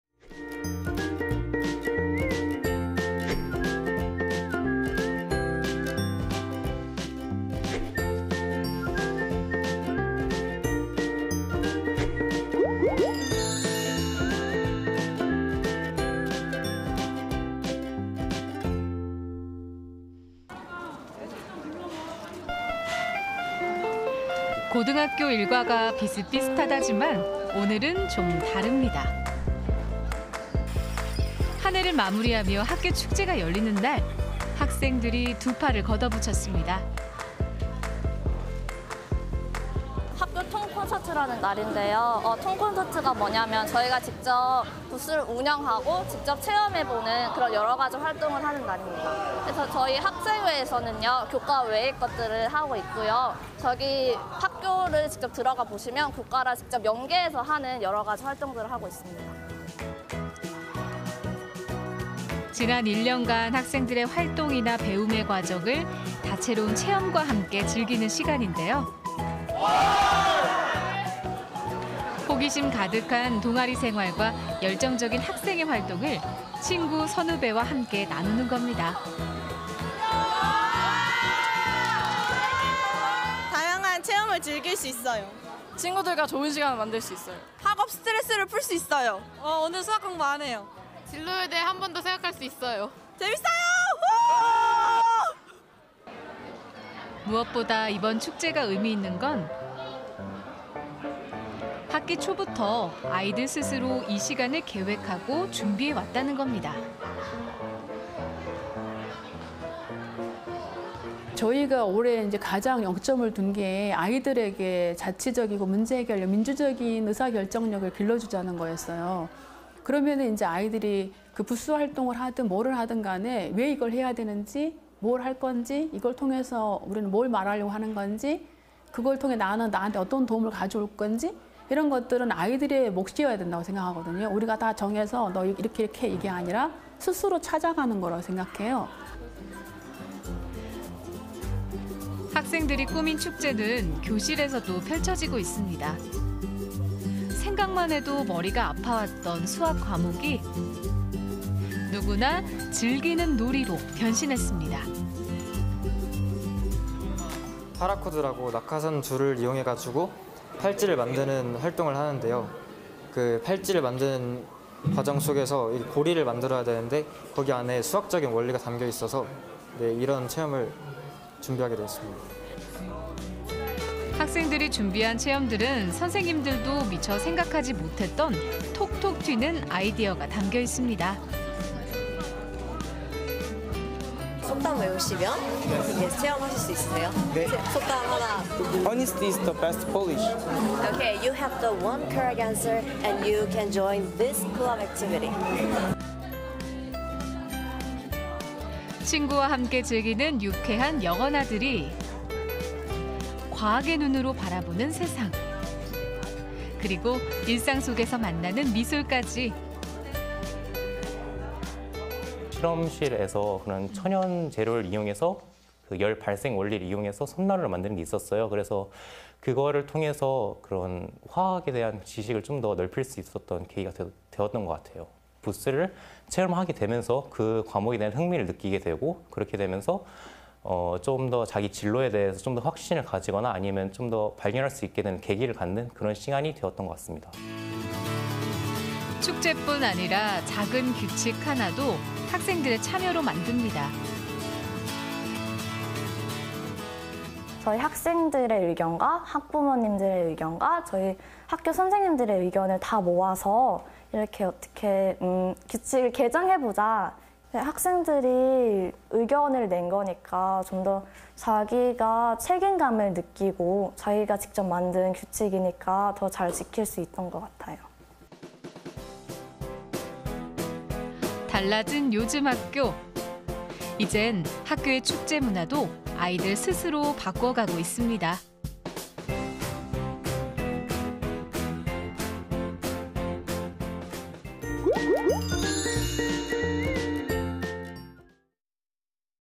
2018 학교가 달라졌어요 6화 축제가 달라졌어요
이제는 수업뿐 아니라 축제도 달라진답니다!!
축제의 모습이 변해가고 있다?
서원고등학교 학생들이 1학기 동안 배운 것을
직접 보여주는 Show time~~!!

동아리활동과 열정적인 학생회활동까지 보고싶으면 우리를 따라와~~ 서원고 통콘서트^^